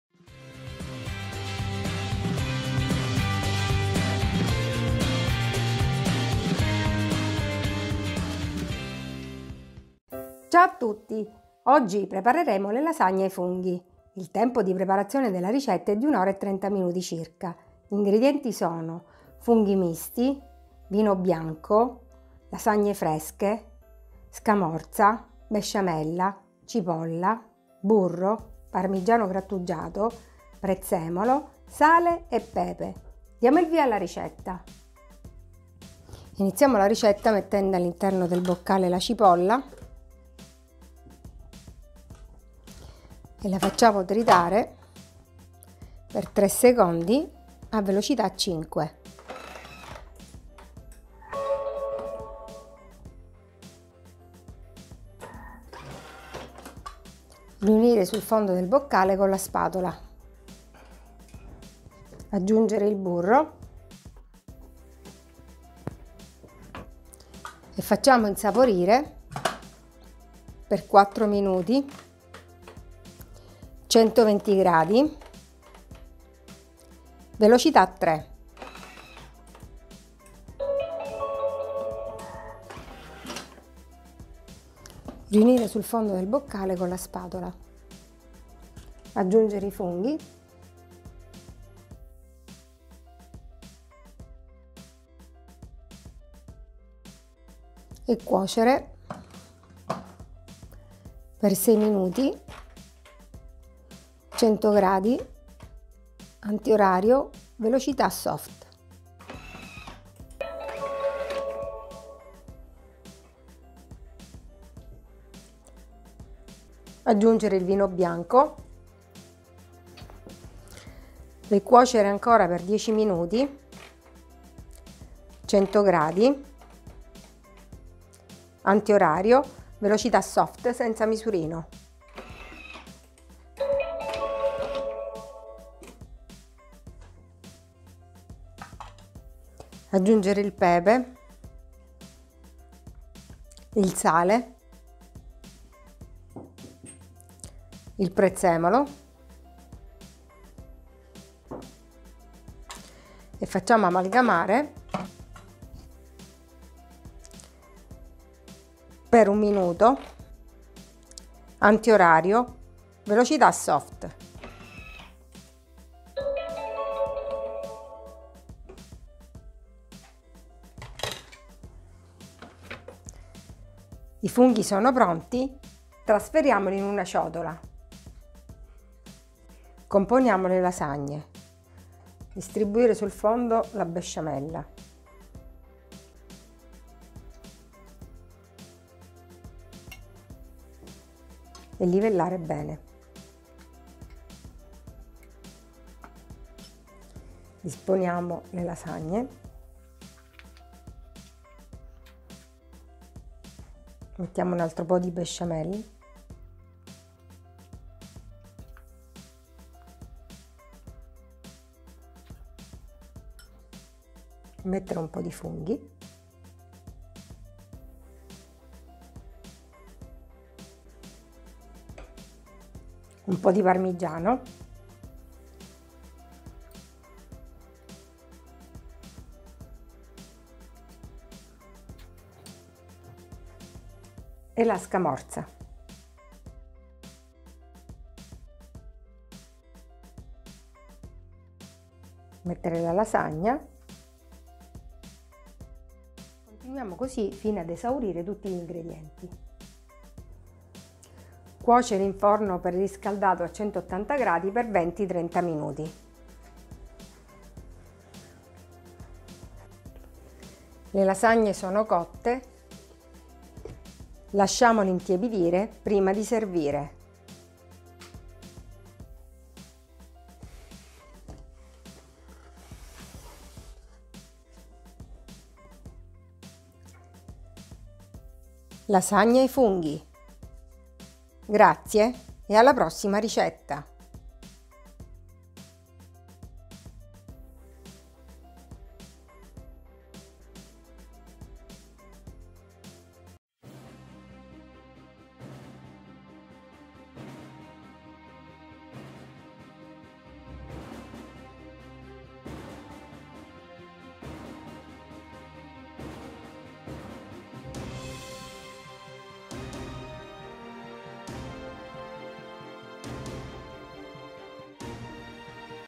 Lasagne ai funghi per bimby TM6 TM5 TM31
Ingredienti:
800 g di funghi misti
70 g di vino bianco
300 g di lasagne fresche
300 g di scamorza
800 g di besciamella preparata con il bimby
1 cipolla
30 g di burro
parmigiano grattugiato q.b.
prezzemolo, sale e pepe q.b.